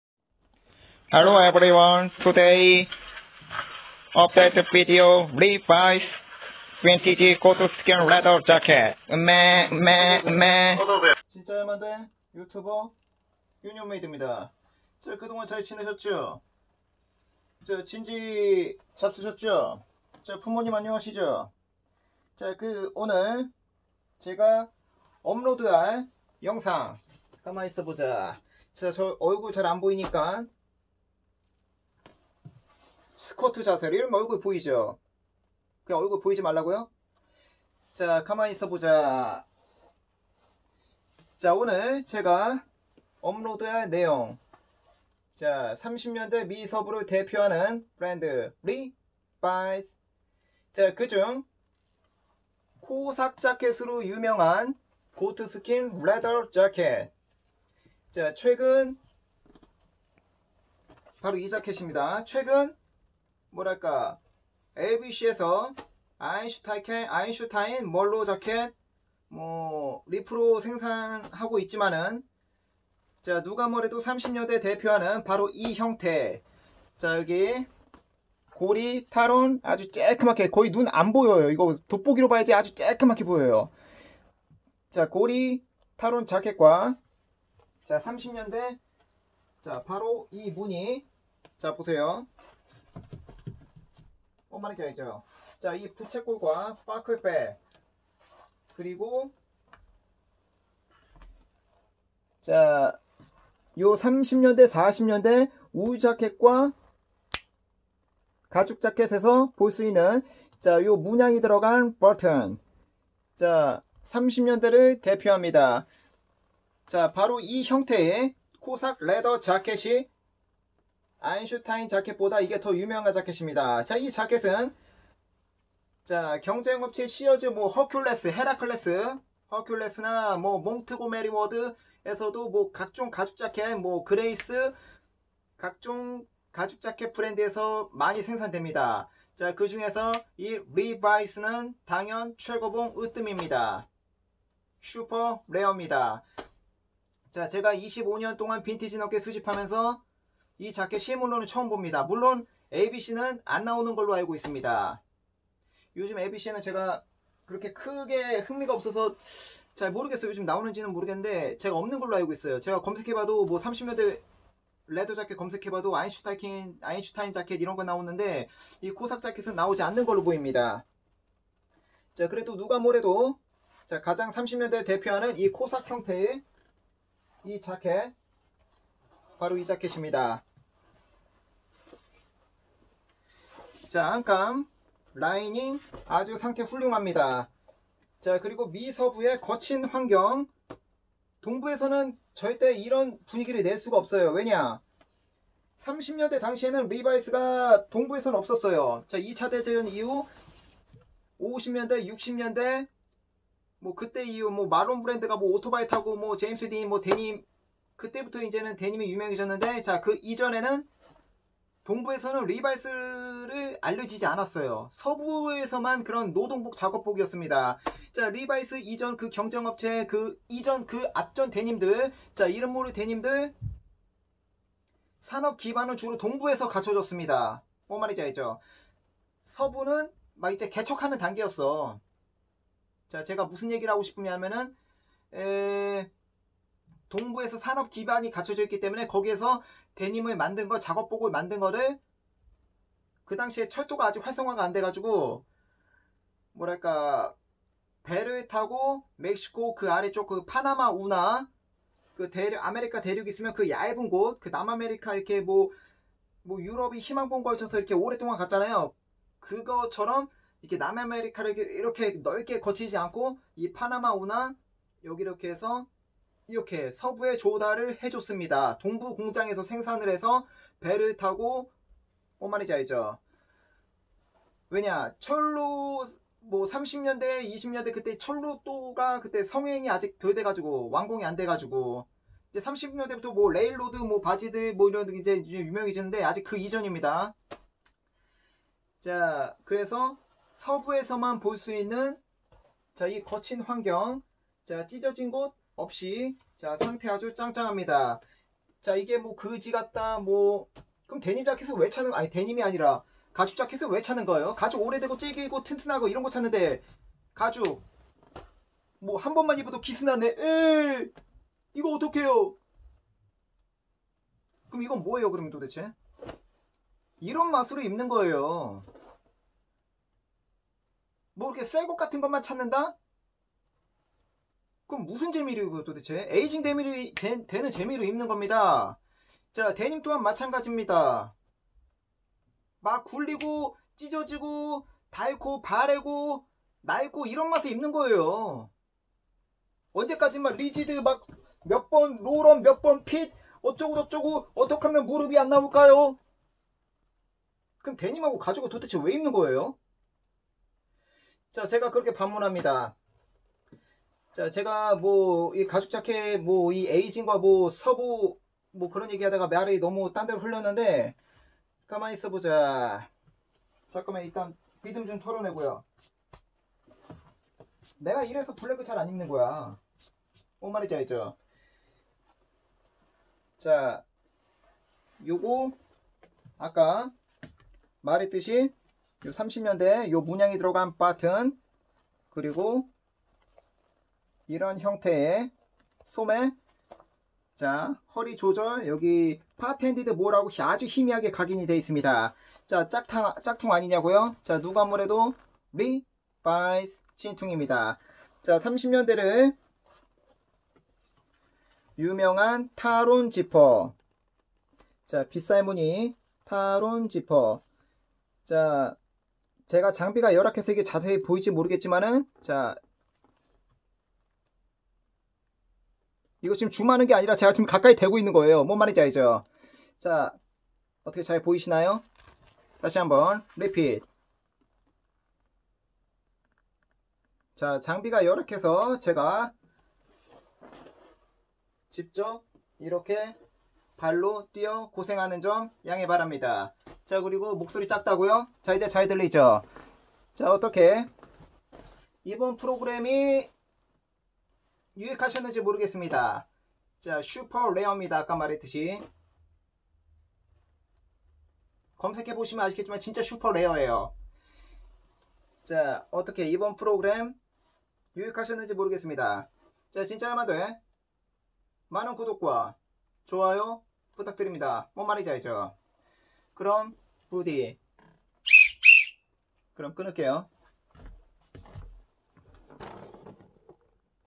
30's Levi's Goatskin Leather Jacket(Super Rare)
30's original levi's goatskin Leather Jacket super rare
30년대를 대표하는 리바이스 코삭 레더자켓 진짜야만돼 에서 업데이트했습니다